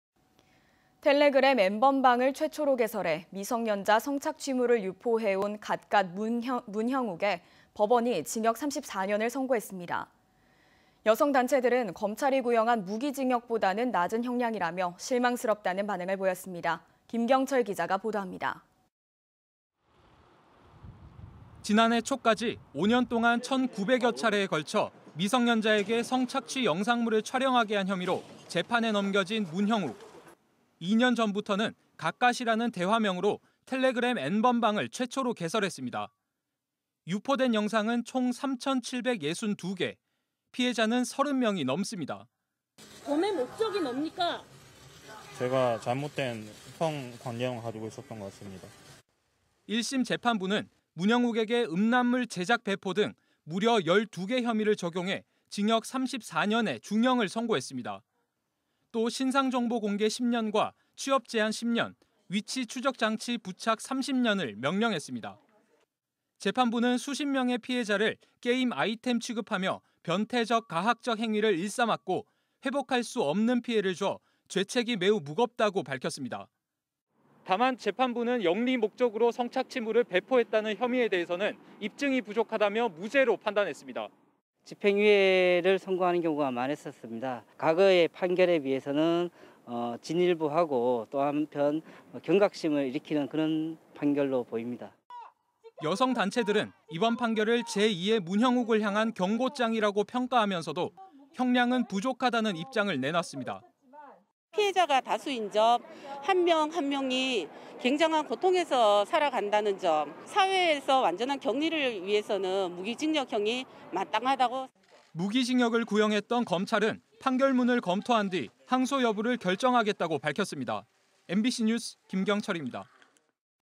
"피해자 게임 아이템 취급"..'갓갓' 징역 34년 선고 (2021.04.09/뉴스투데이/포항MBC)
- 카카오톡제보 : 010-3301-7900
- 카카오톡ID : PHMBCJEBO